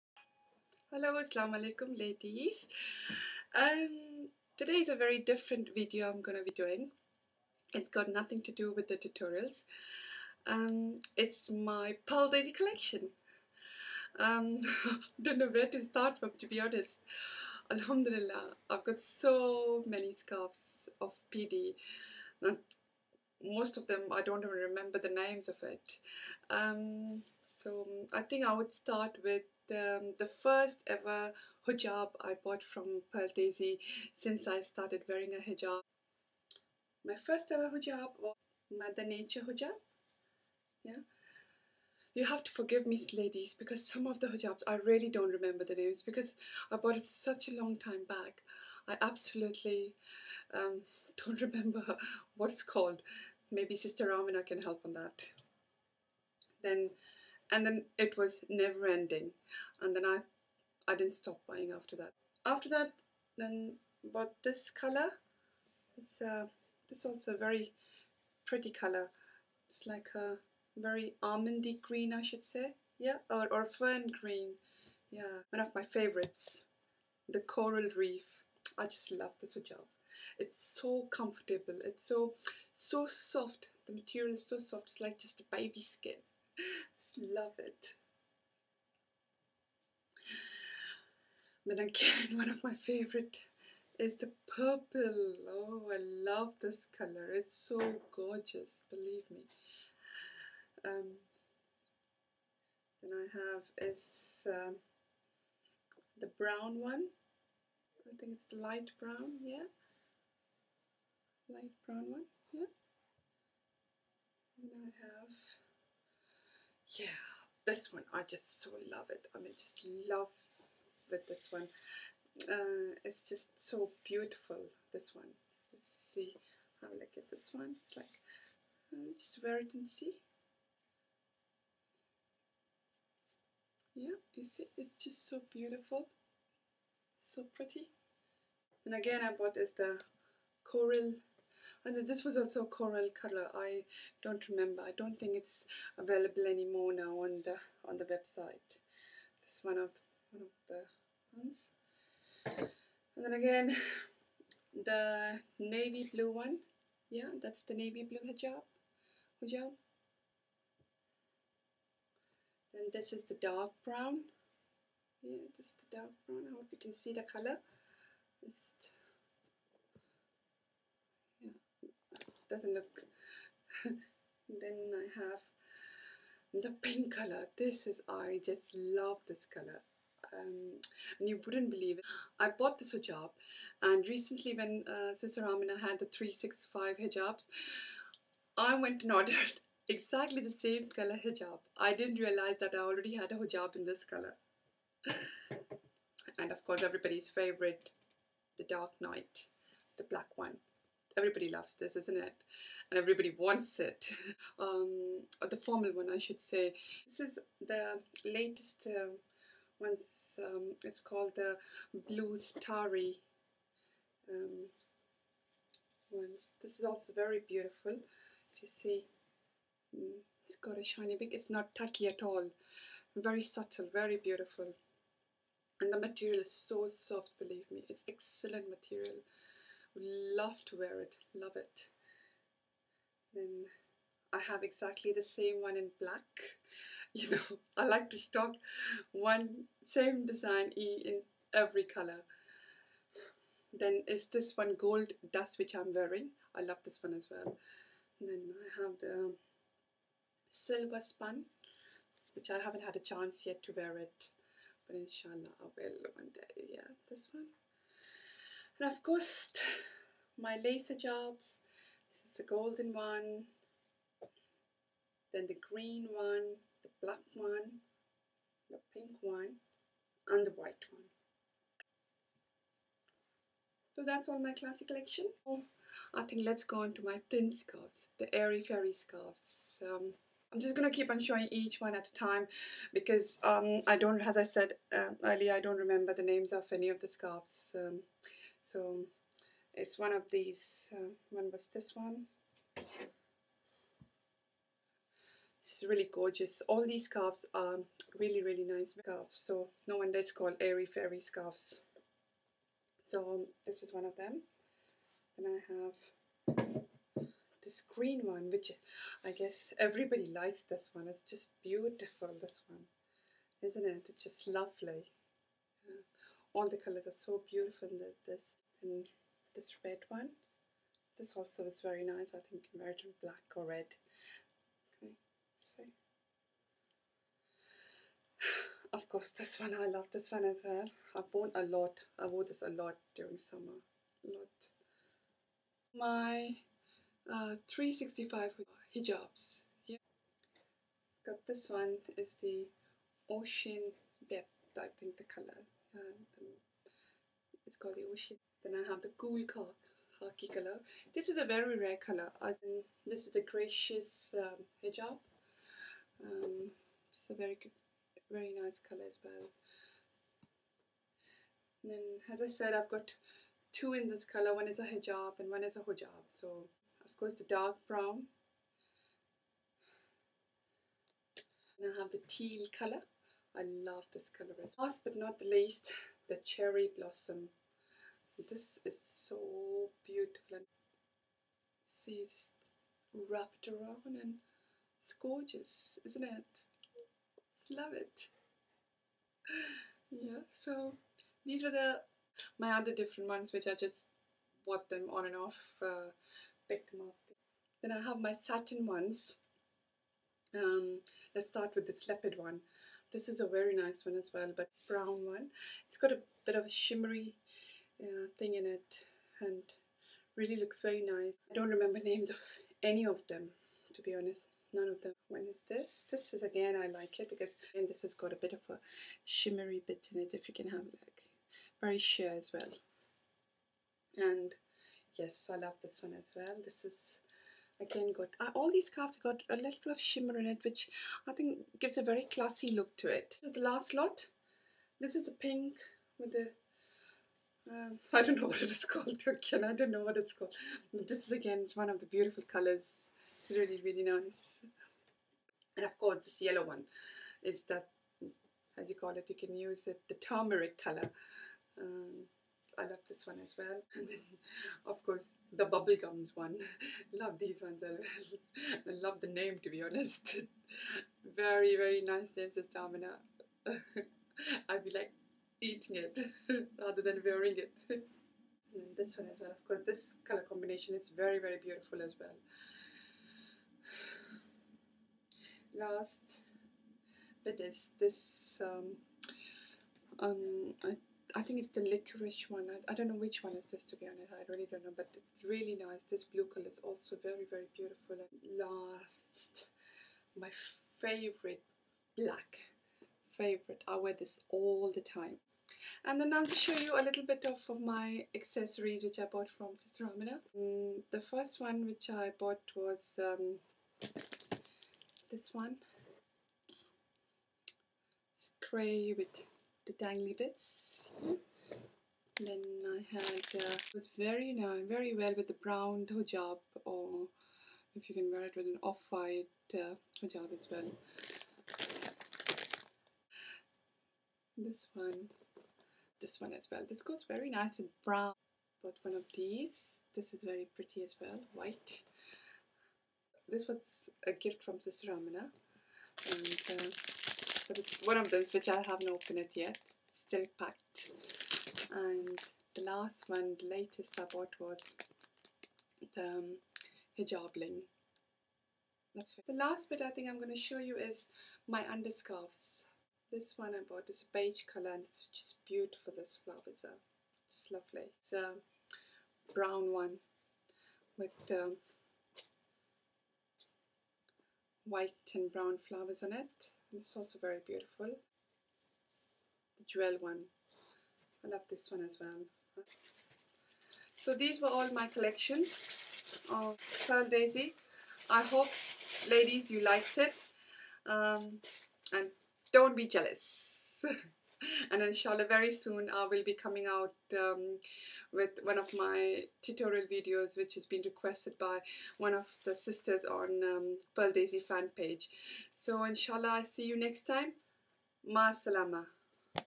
Pearl Daisy Collection
Pearl Daisy haul Part1
In total i have more then 50 scarves  only from PD...